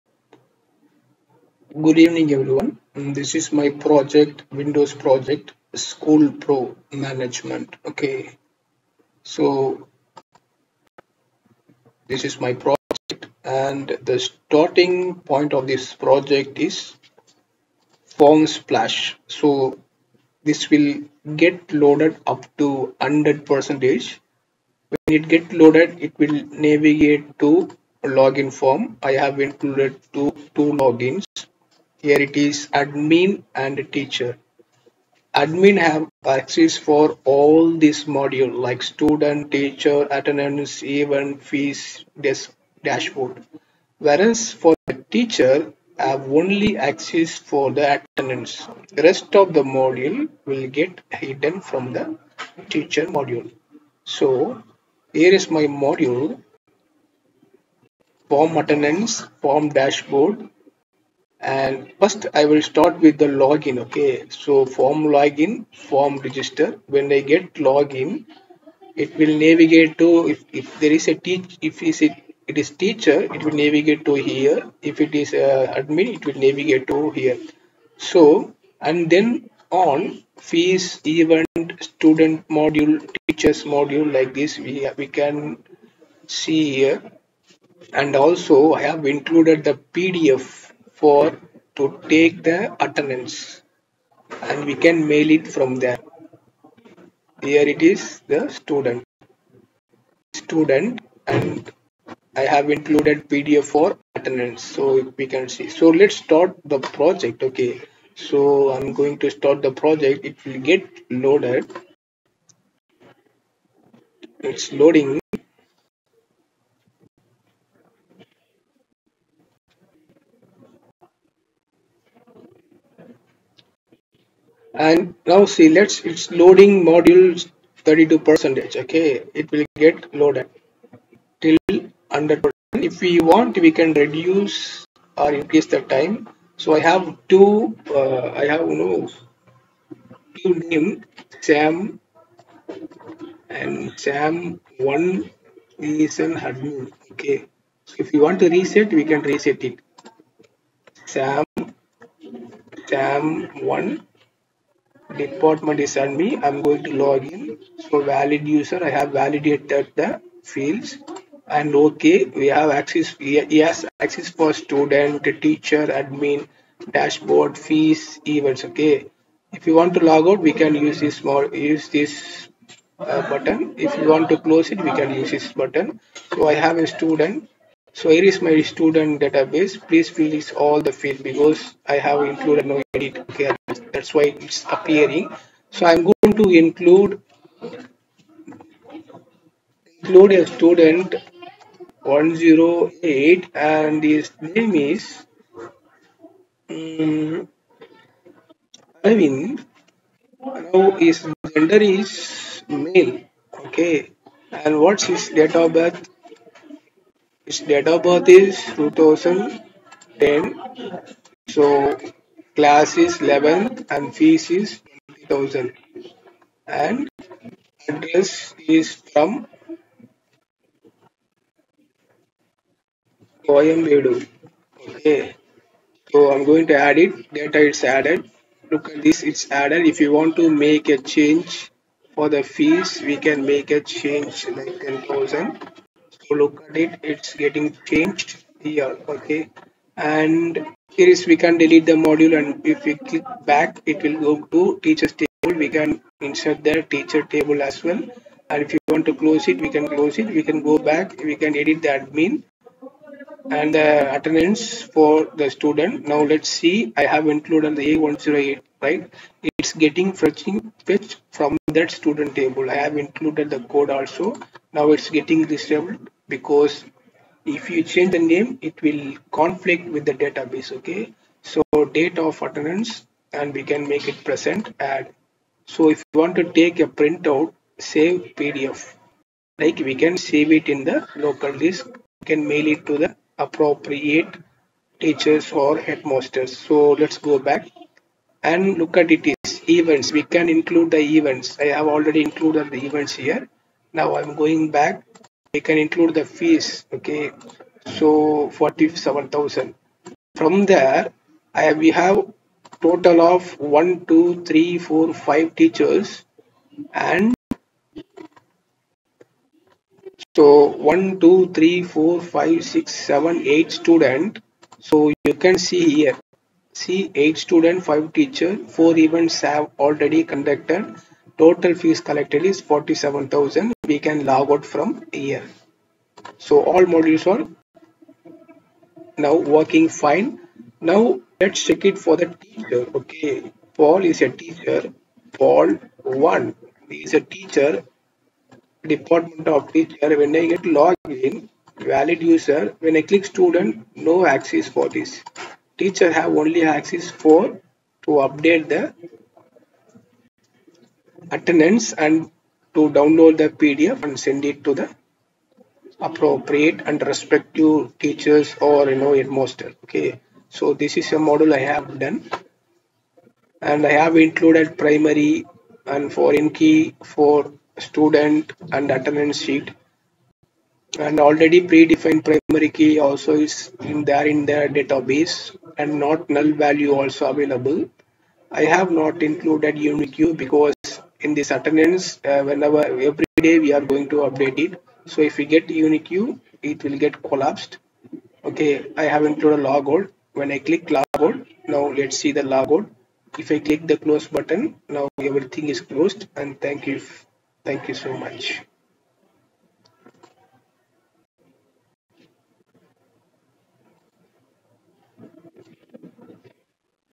kaashiv infotech student review - Visual C# with WinForms Projects for Beginners
In this kaashiv infotech student review video, you’ll learn Visual C# with Windows Forms (WinForms) through simple, practical projects — perfect for beginners who want to start building desktop applications using Microsoft Visual Studio.

🎯 Visual C# with WinForms | Step-by-Step Projects for Beginners

Project Cost - Rs.5000 - Visual C# with WinForms Step by Step Projects for Beginners

School Management Windows Project Cost : ₹ 5000